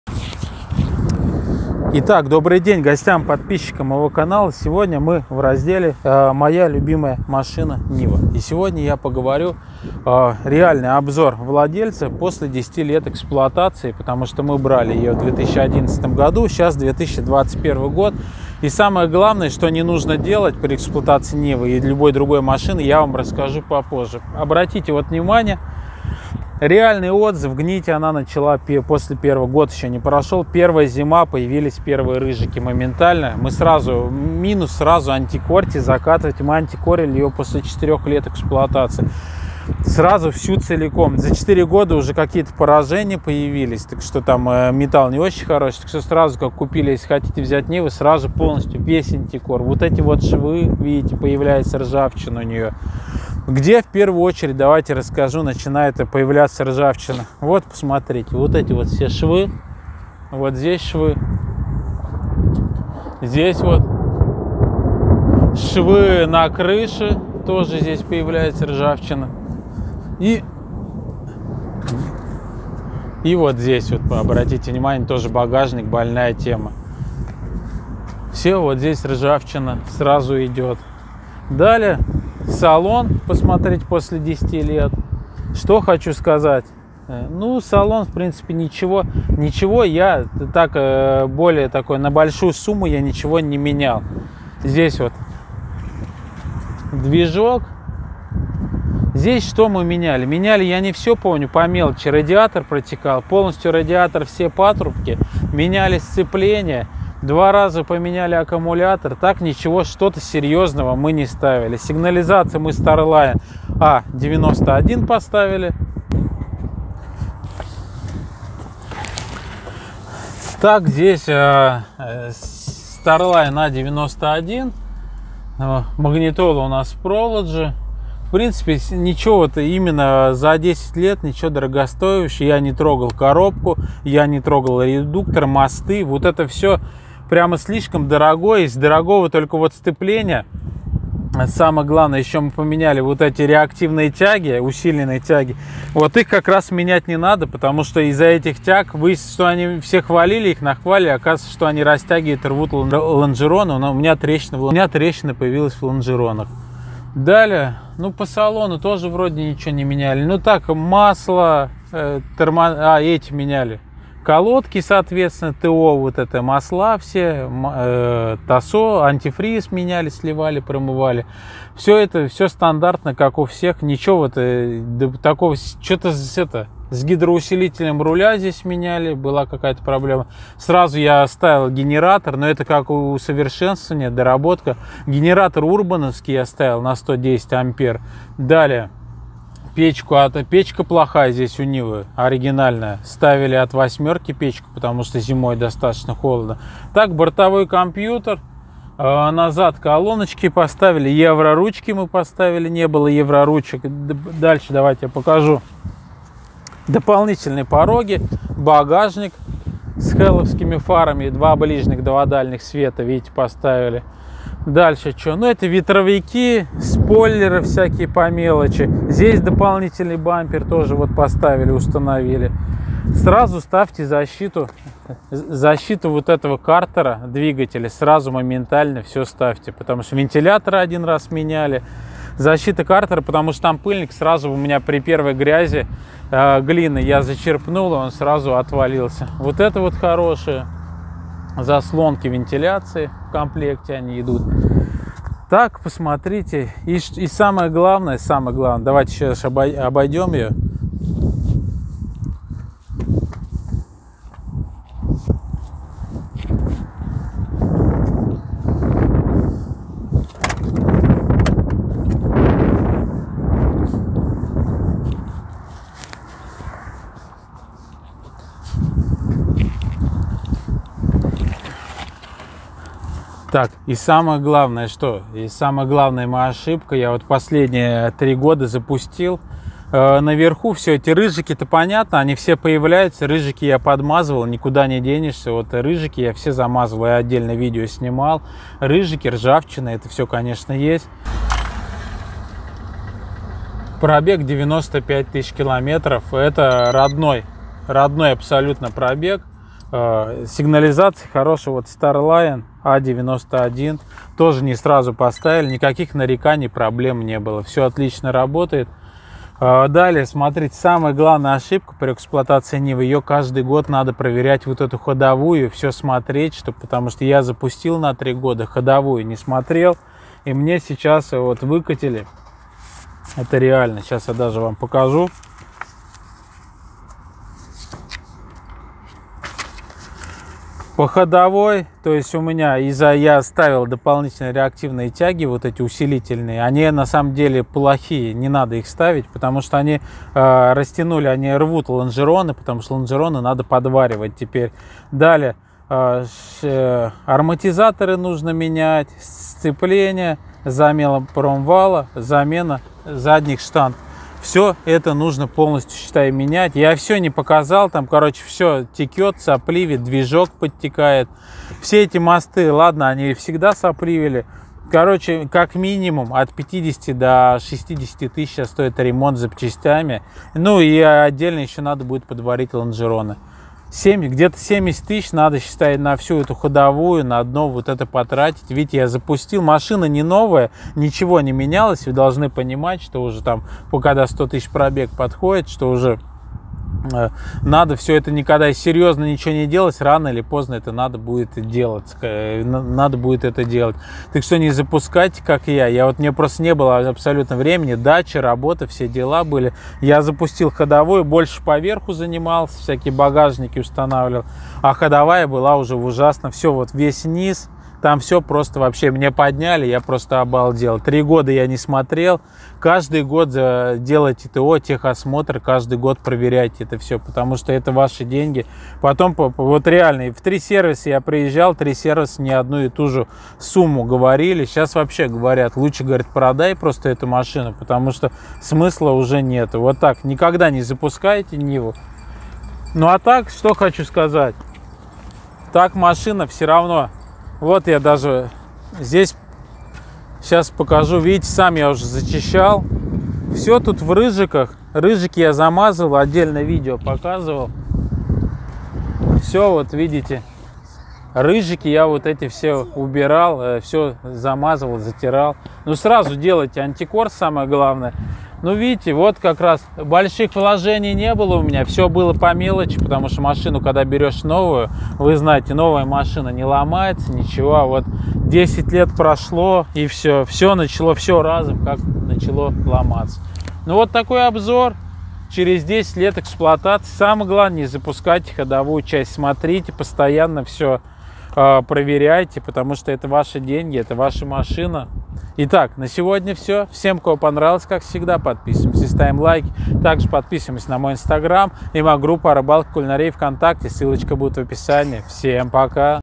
Реальный обзор НИВЫ ВАЗ 2131 после 10 лет. Важно не запускайте НИВУ, УЧИМСЯ НА МОИХ ОШИБКАХ.
В этом видео я покажу НИВУ после 10 лет эксплуатации.